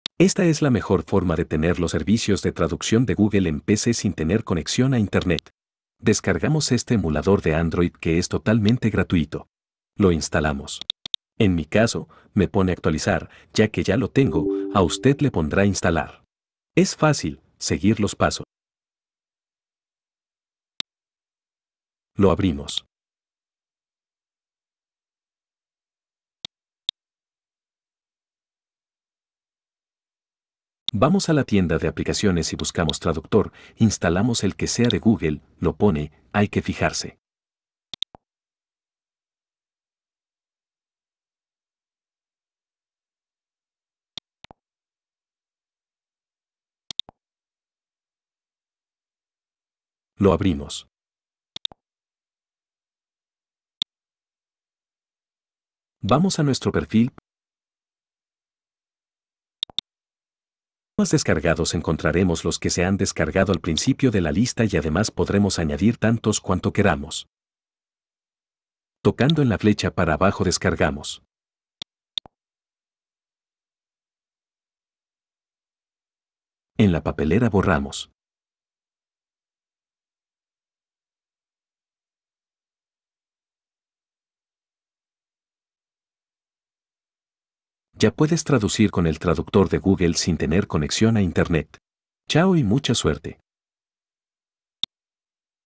Traductor De Google en Windows 11 sin conexión a internet
Cómo usar el traductor de Google sin conexión en Windows
Descargar idiomas para utilizarlos sin conexión